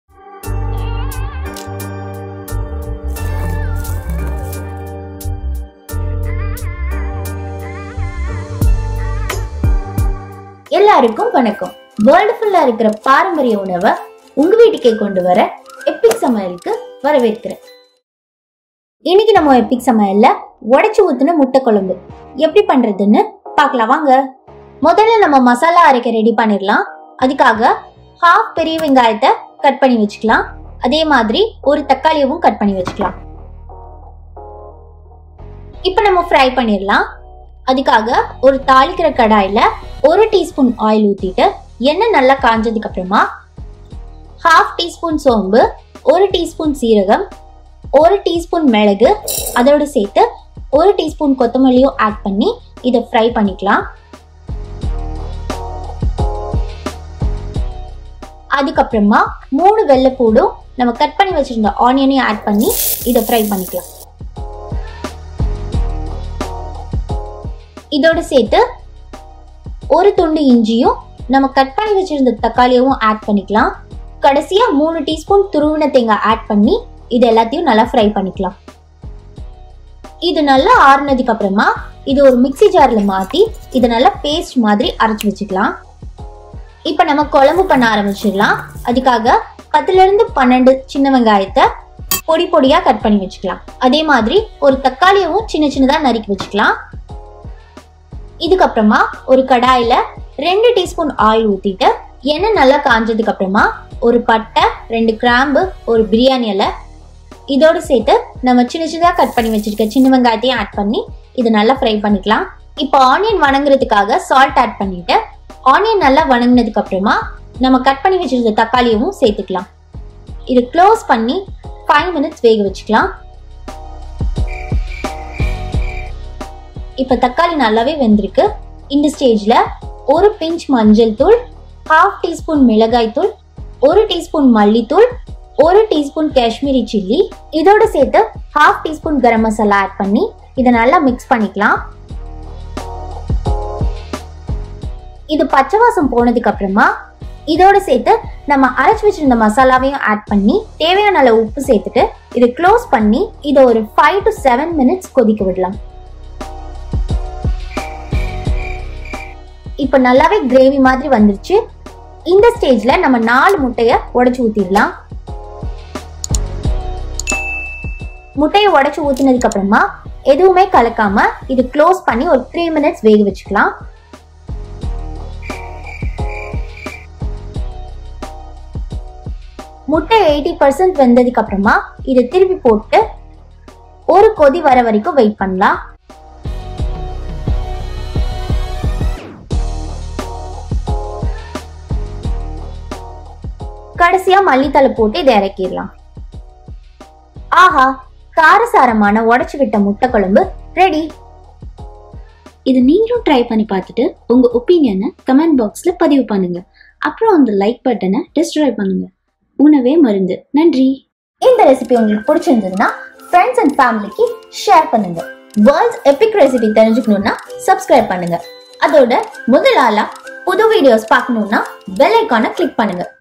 Unique Broken Egg curry | Udacha Muttai kulambu in Tamil | Craked Egg curry recipe
How to make Udacha Muttai kulambu in Tamil also know as Udacha egg gravy, one of the easiest egg curry out there, Udacha egg curry takes much less time to prepare and one of the quickest goto egg curry. Udacha means 'cracked' egg, as the name suggests we use a few cracked eggs to make this curry. Udacha muttai kulambu is rich in spices so its aroma makes you eat more, Udacha egg curry goes well with white rice and wheat chapati, Enjoy our Udacha muttai kulambu recipe!

Udacha Muttai kulambu in Tamil | உடைத்து ஊற்றிய முட்டை குழம்பு | Egg curry recipe | Udacha egg gravy